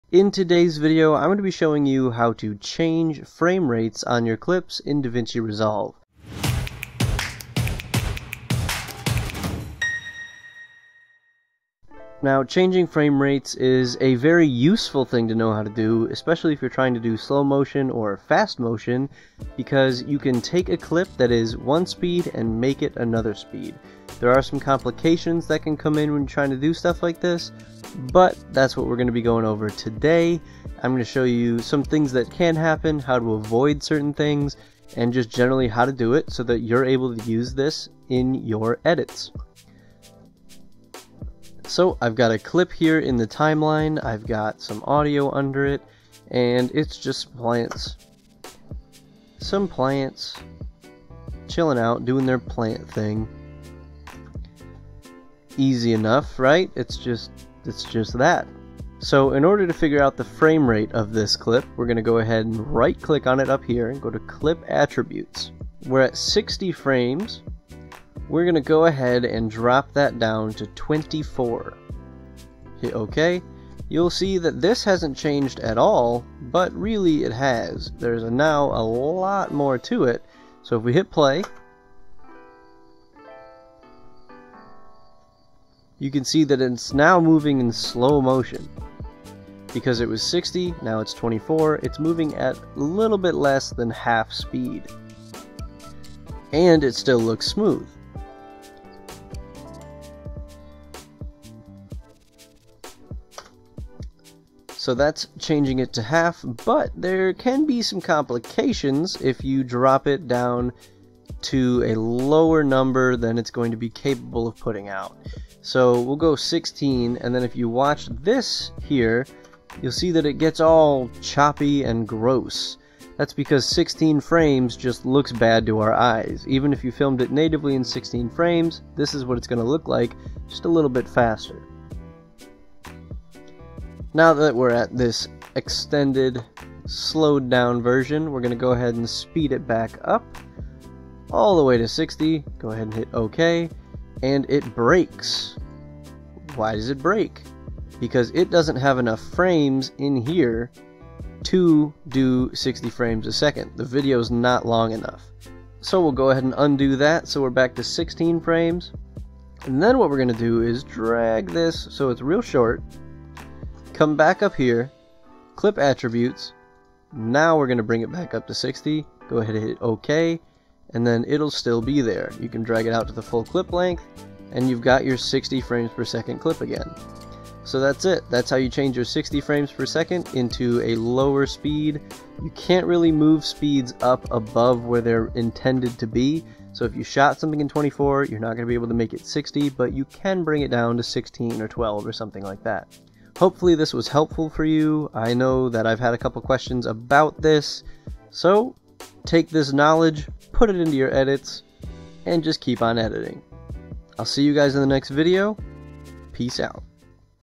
Change Your Frame Rates in Davinci Resolve 16! (Slow Motion)
Have a 120 or 60fps clip you can't seem to slow down? Or a 24fps clip you are trying to stylize? In this video, I teach you how to change your frame rates in Davinci Resolve 16. This will allow you to create slow motion clips from footage shot in 120, 60, or really any frame rate.